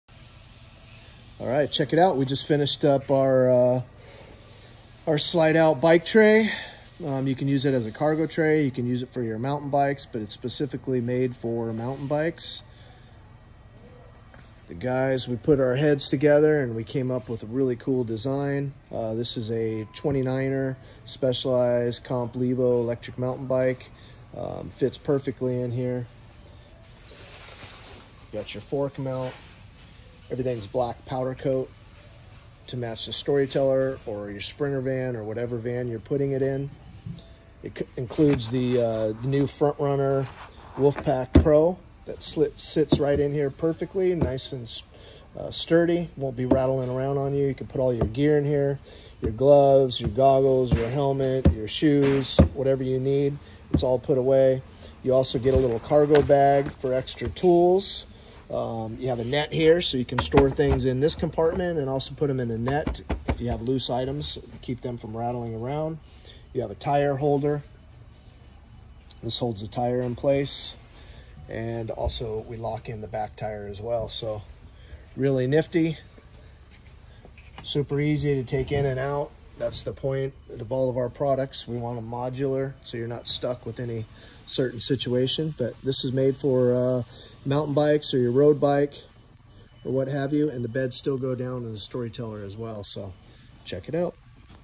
BTSS - Bike Tray Storage System
The Ultimate Bike Tray Slide Out. Made for the Storyteller Overland Mode, Winnebago Revel 4x4, Ford Transits and All Sprinters.